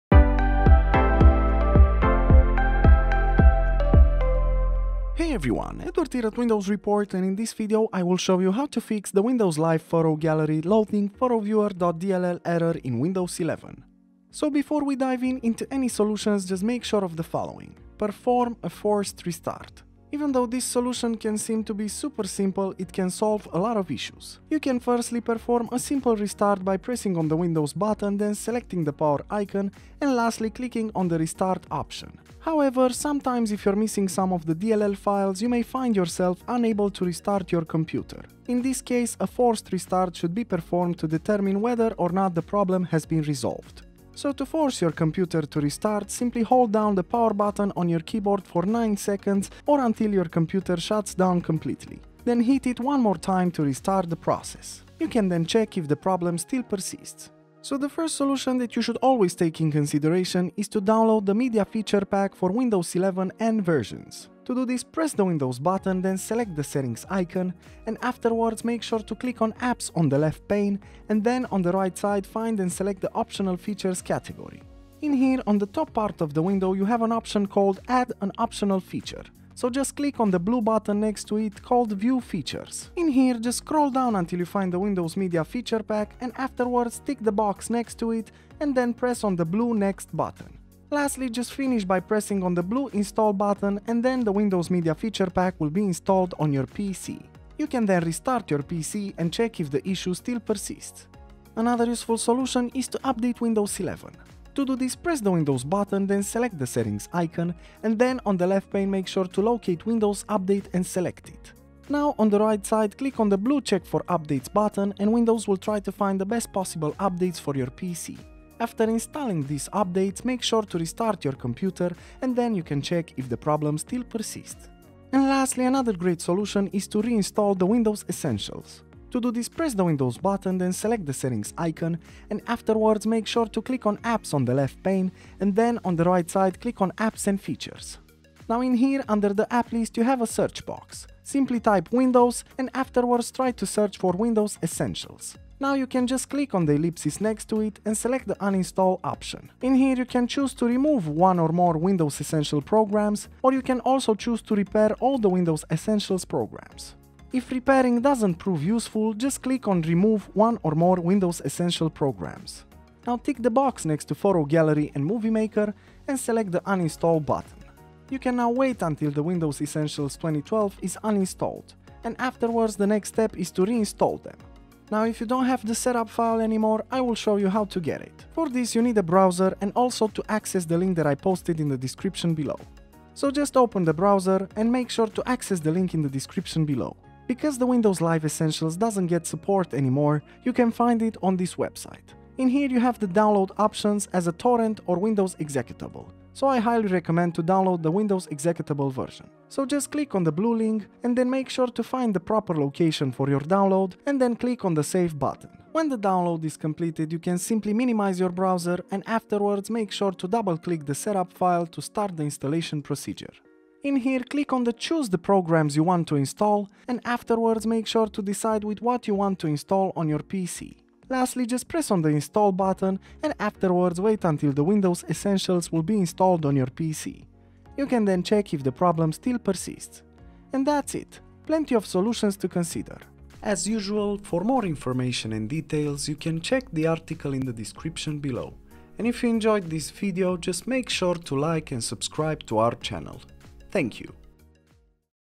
FIX: Windows Live Photo Gallery loading photoviewer.dll error
| ⚙️Try Fortect to fix and optimize your PC like a Pro

Interested in how to FIX: Windows Live Photo Gallery loading photoviewer.dll error in Windows 11?

0:00 - Intro
0:57 - Download Media Feature Pack for Windows 11 N
1:41 - Update Windows
2:05 - Reinstall Windows Essentials 2012